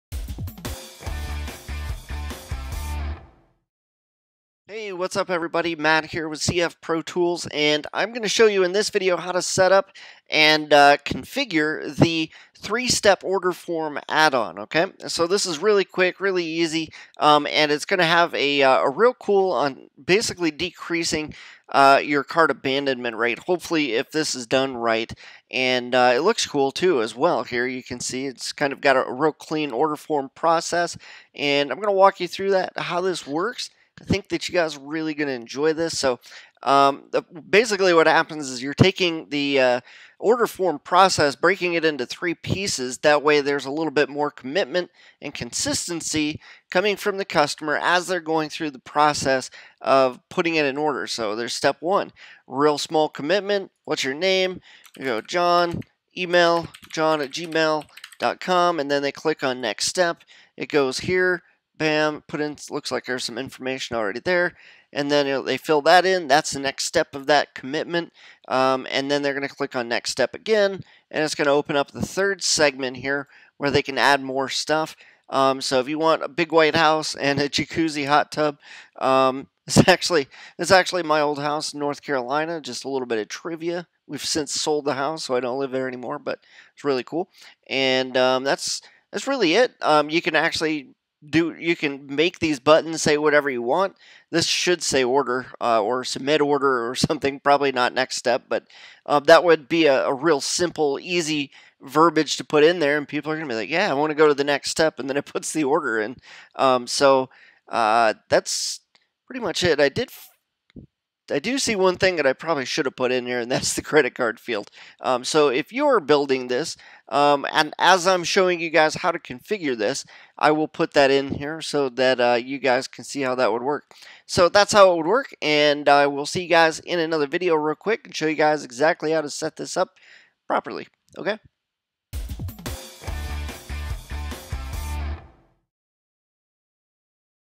How To Create A 3 Step Order Form In ClickFunnels :: Demo
-- If you want to create a more dynamic and customizable order experience for your customers, you can actually build a 3 (or even 4) step order form in ClickFunnels on an Order Page using this amazing add-on.

As you progress through a series of rows that request information from the customer, additional rows for the order process will unveil themselves giving the customer a new and sleek experience while ordering their products and services from you.

-- Get started today to see how you can get higher conversions with your existing ClickFunnels pages.